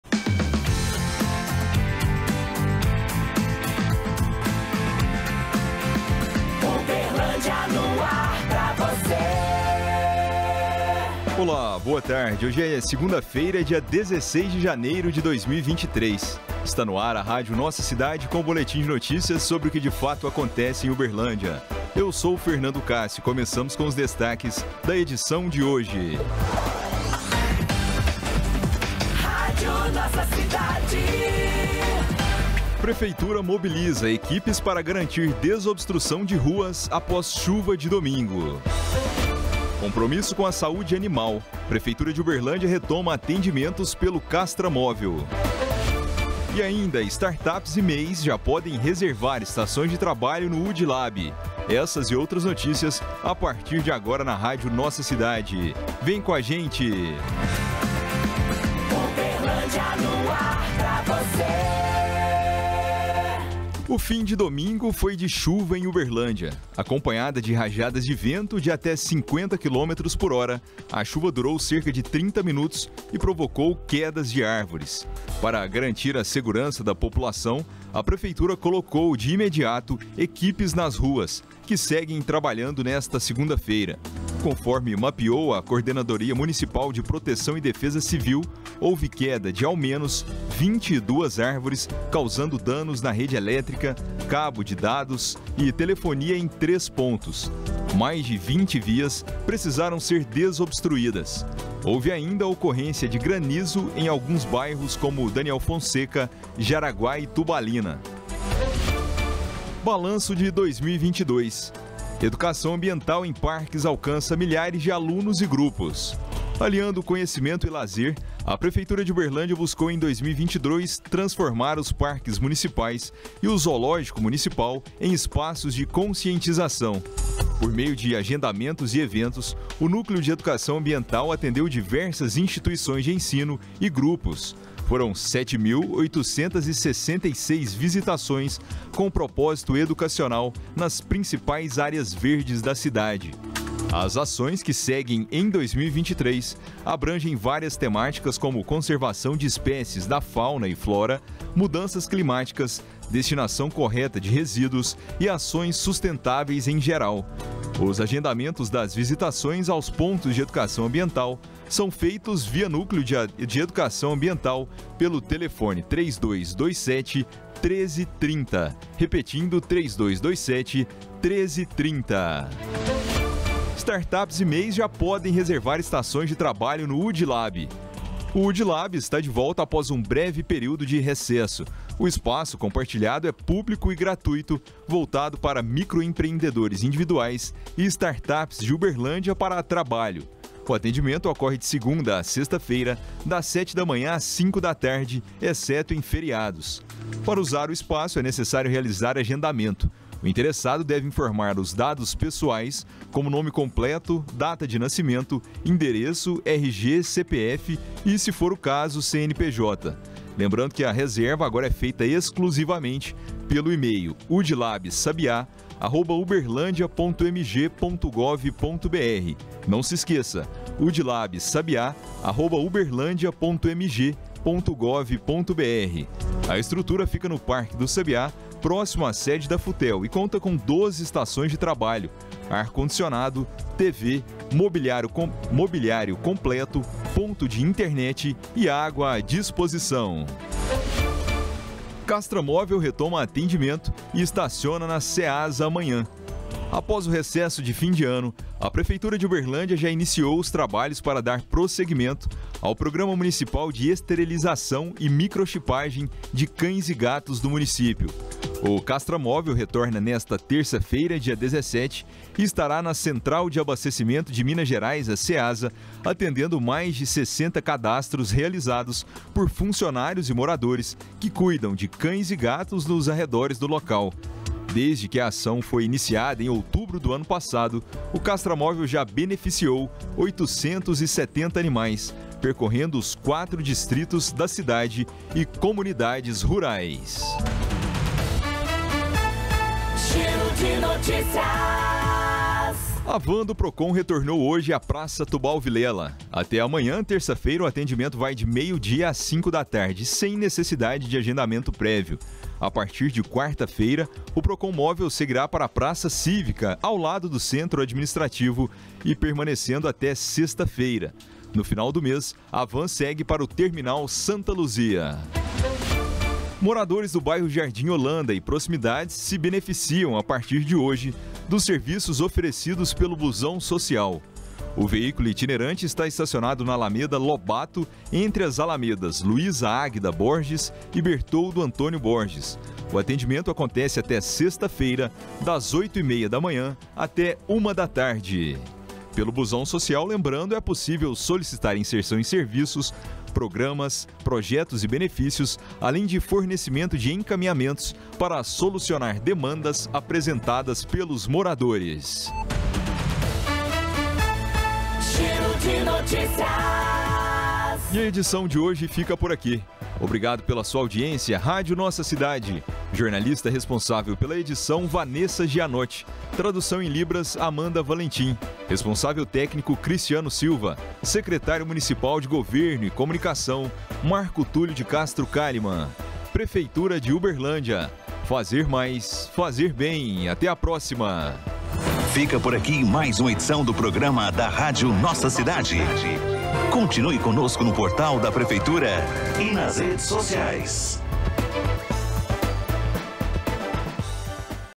#865: Prefeitura mobiliza equipes para garantir desobstrução de ruas após chuva de domingo
Compromisso com a saúde animal prefeitura retoma atendimentos pelo castramóvel, Startups e MEIS já podem reservar estações de trabalho no UDILAB